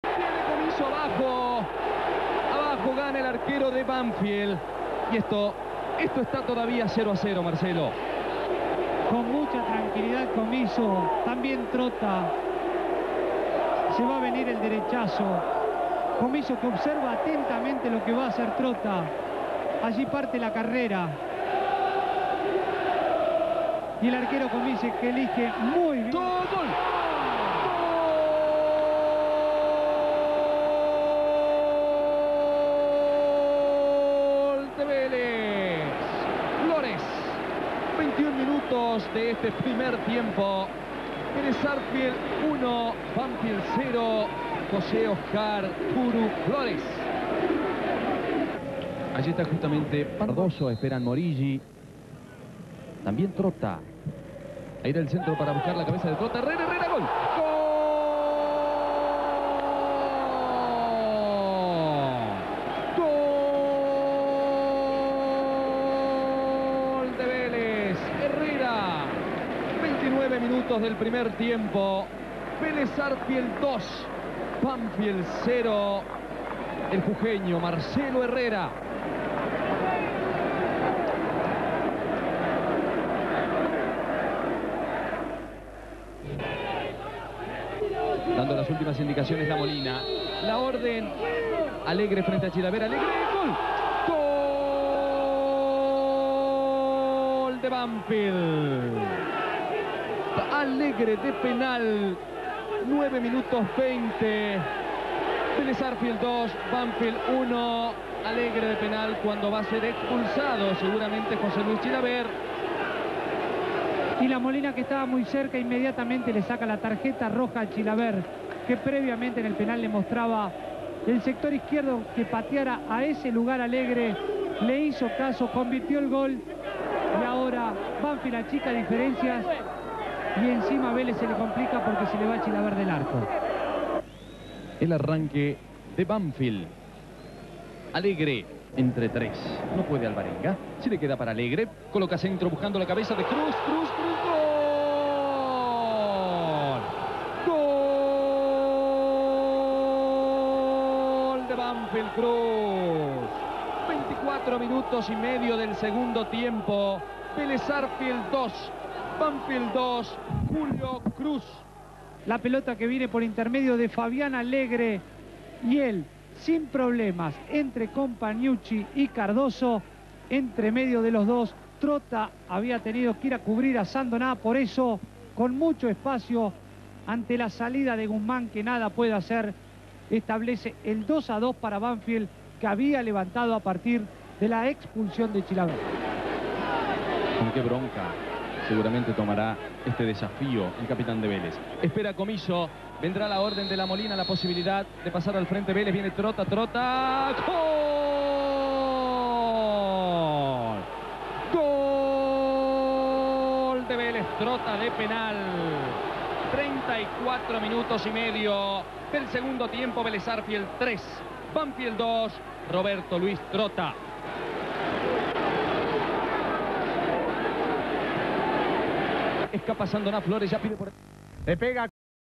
10-3-1996 (Clausura) (1°F) Velez:3 vs Banfield:2
Goles:Flores-Herrera-Alegre-Cruz-Trotta.